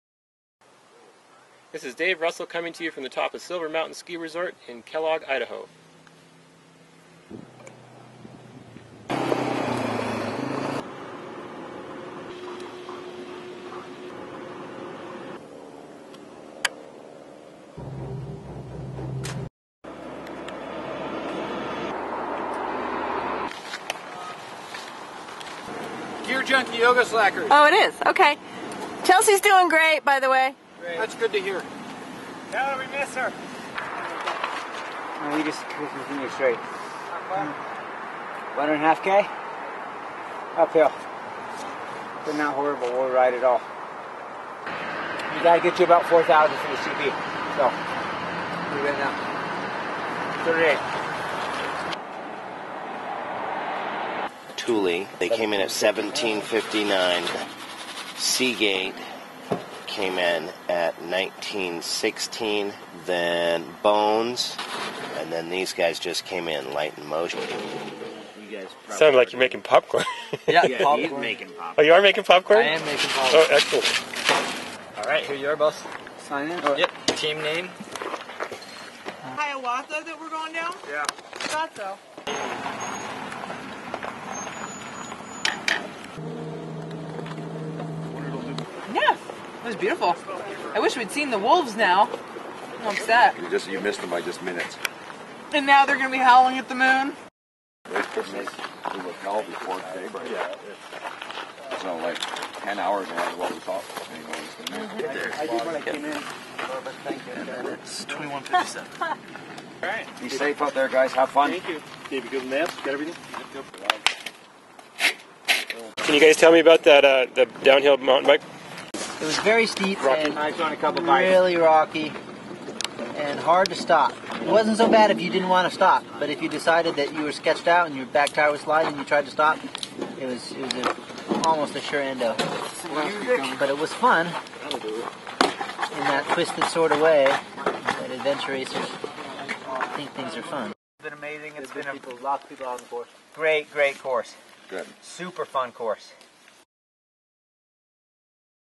Day 1 - Bike into the night
Under a beautiful full moon, the racers climb to 4,800' and TA-1 for the transition to trek. At this point, only 6 teams had arrived. Fast moving Team Thule already had built a 1hr lead over Seagate with savvy navigation thru the ski area.